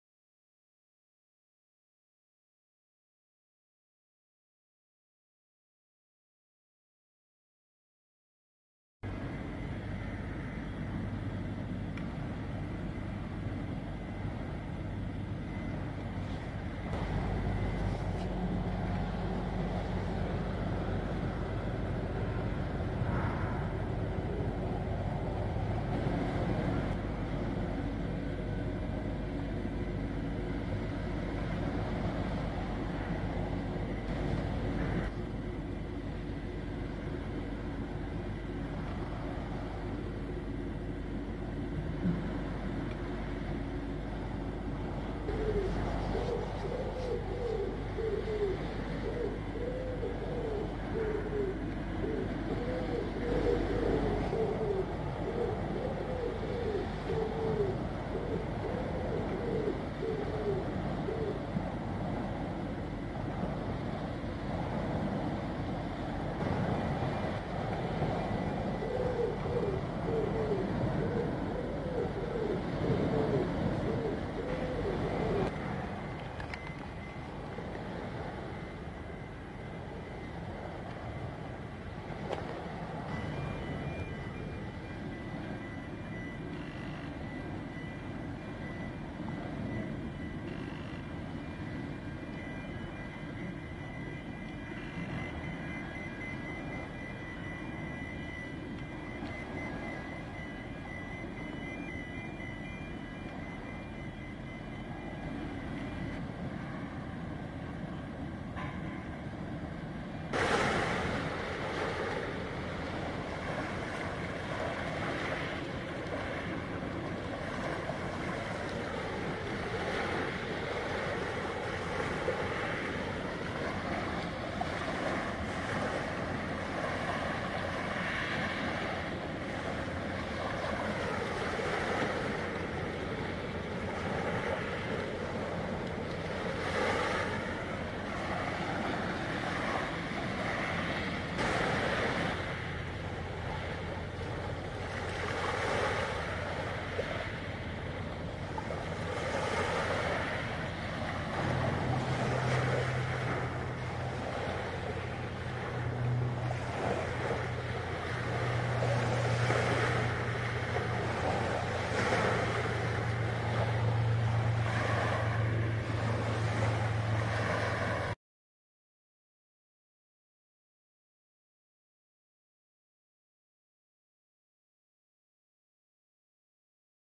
ice-class VUOKSI MAERSK departing the port of felixstowe 11/7/19
200m length ice-class vuoksi maersk departs the uk port of felixstowe and heads off for her next stop of kotka in finland.